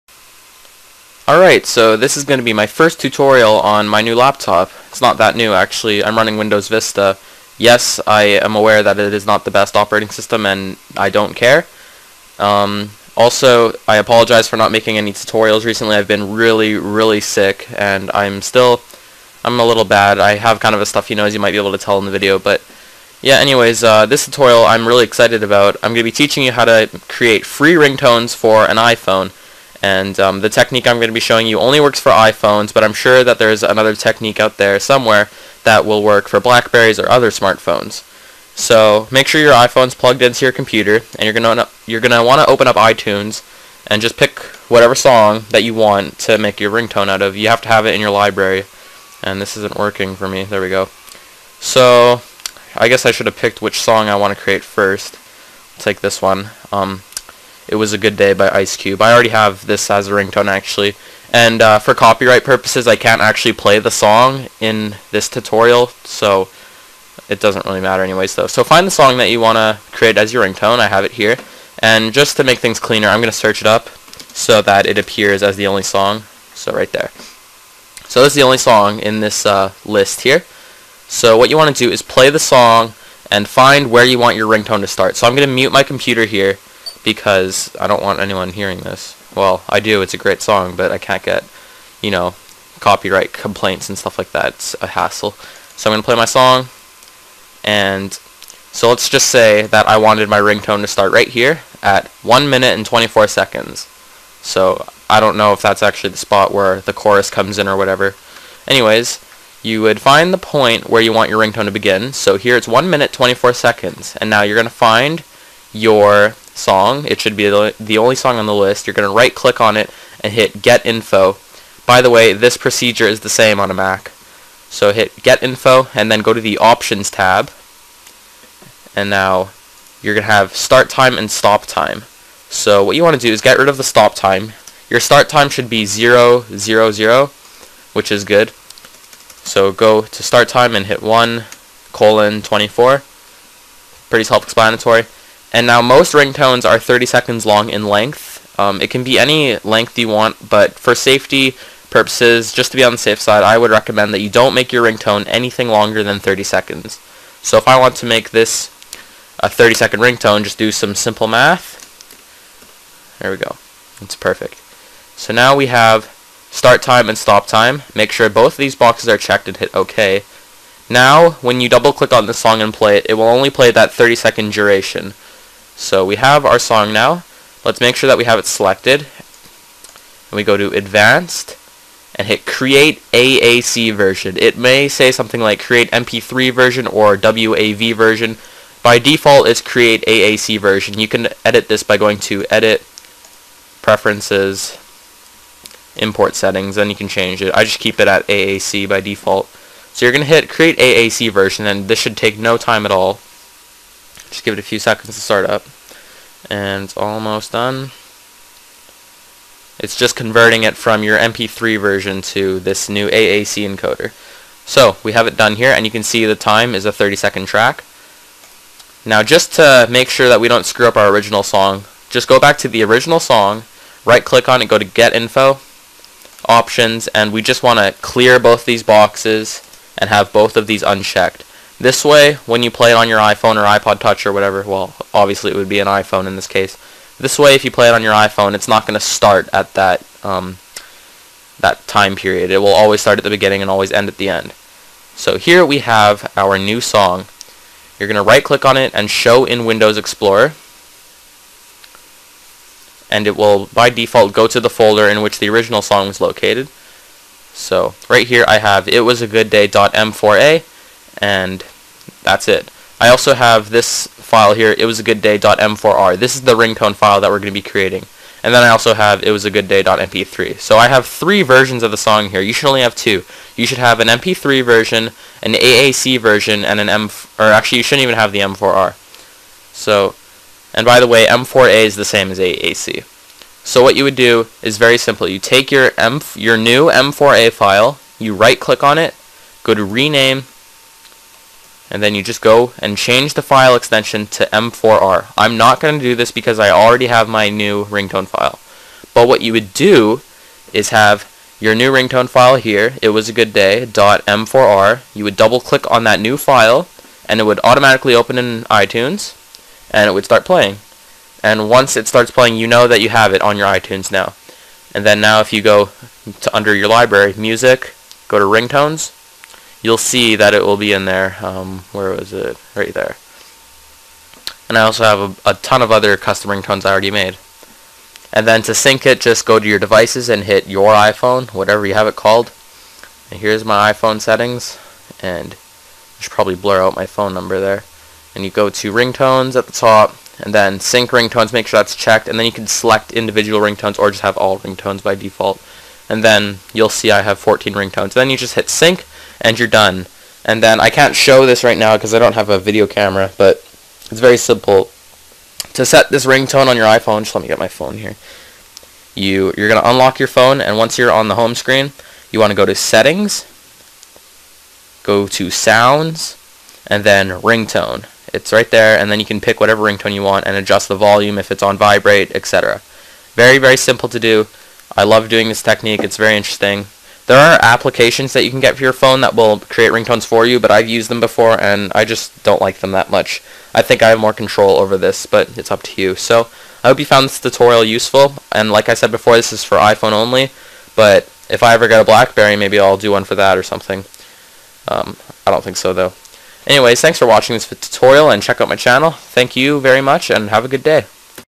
How to create custom ringtones for iPhone!
Sorry for the quality on this one boys, need to screw around a bit more with some stuff on this computer.